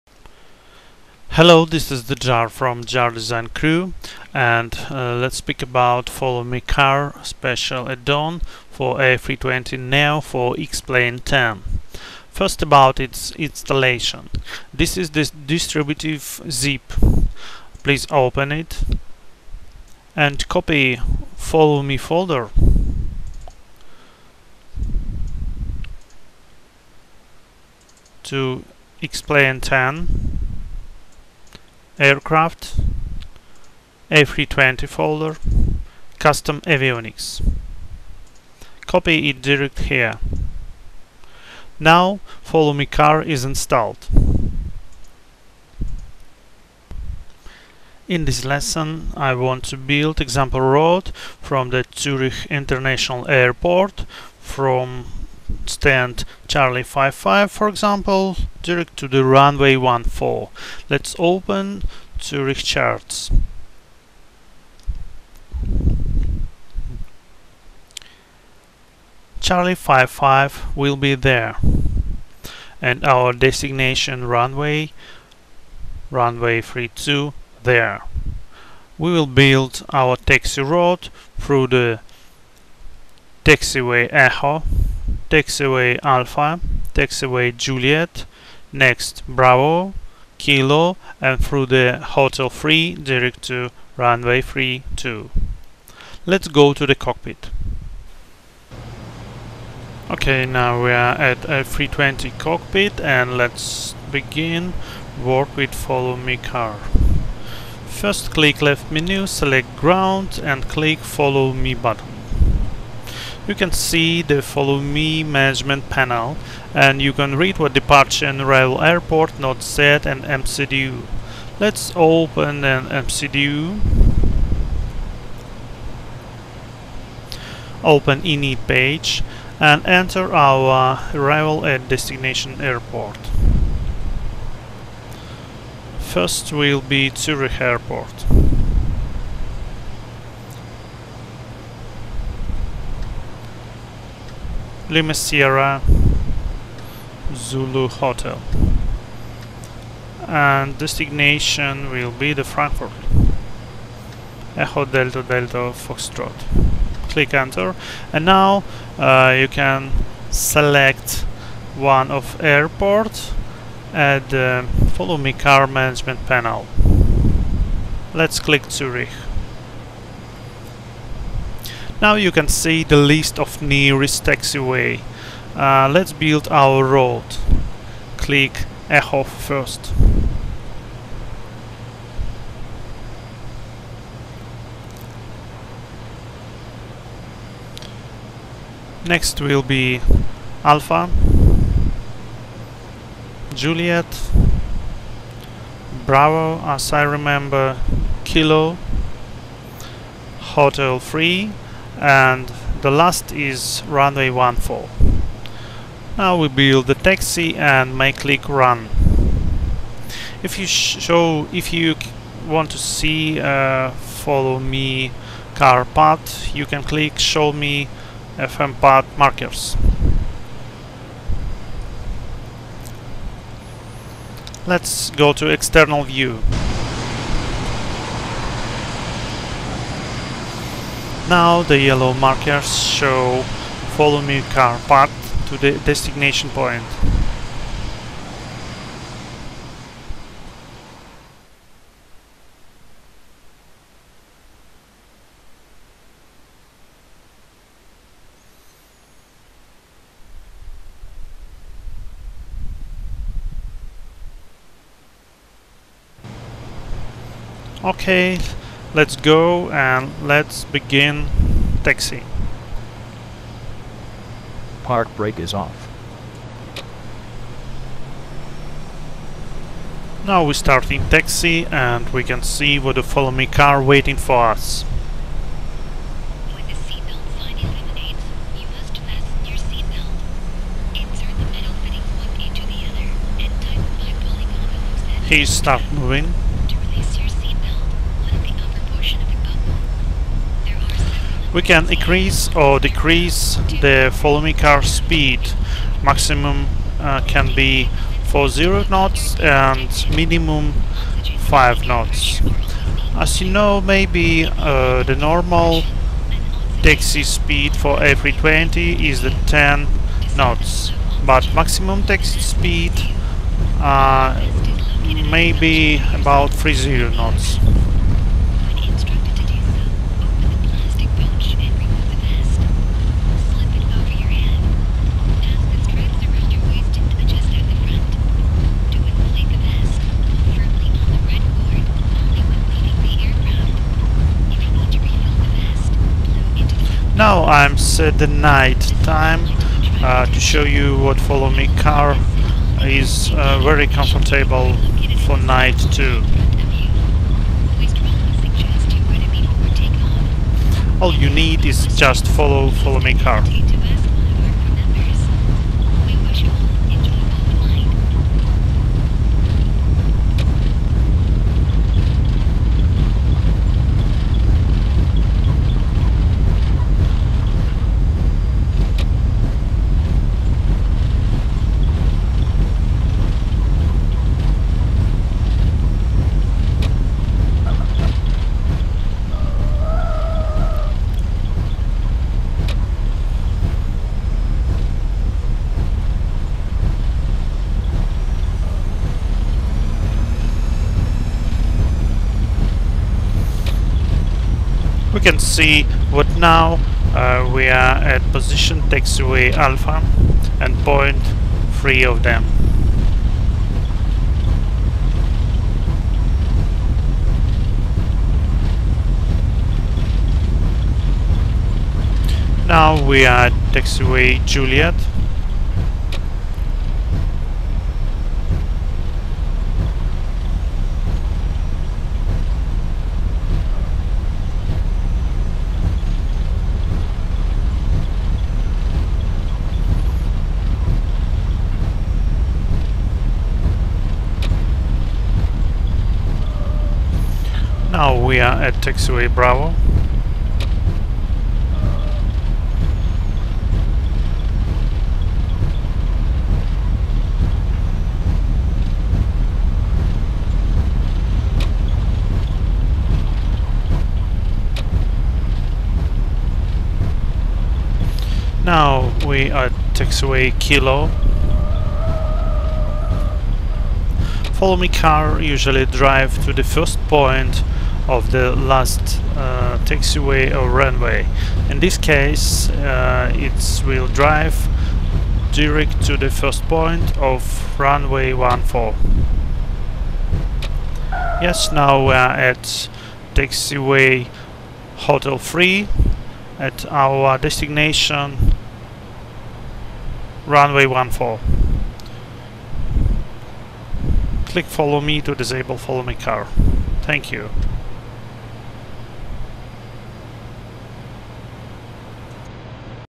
How to install and use follow me car for a320neo by JARDesign
How to install and use follow me car addon for a320neo by JARDesign